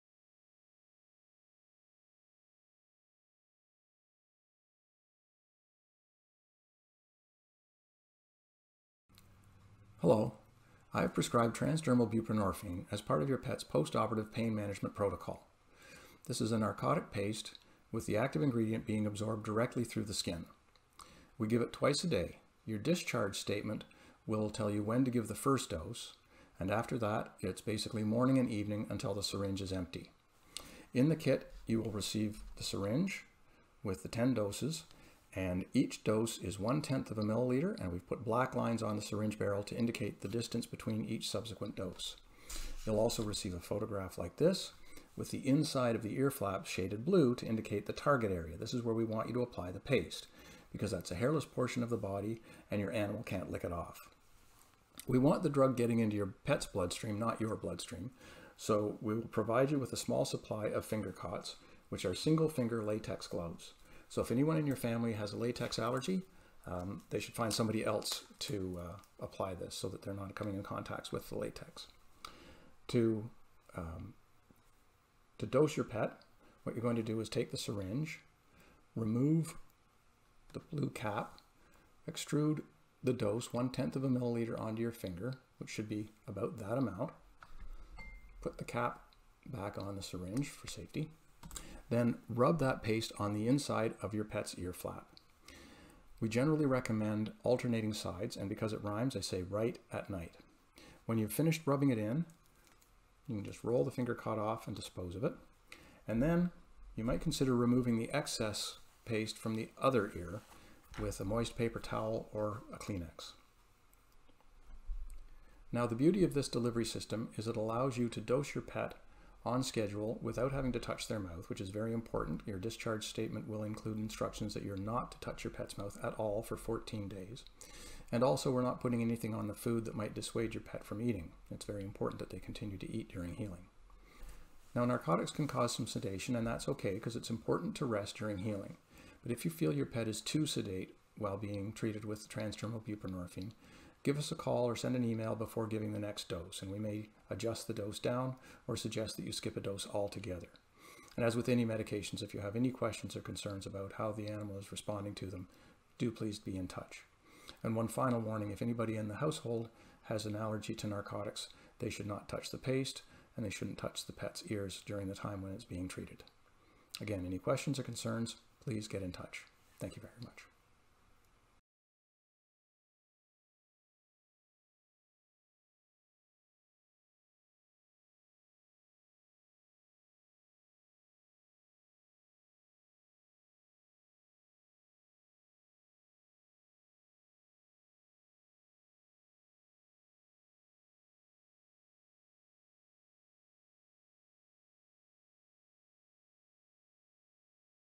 Transdermal buprenorphine for post-operative pain management in cats and small dogs.
This is an instructional video for clients of Hale Veterinary Clinic to whom we have dispensed transdermal buprenorphine as part of the post-operative pain management protocol for their pet (cat or small dog).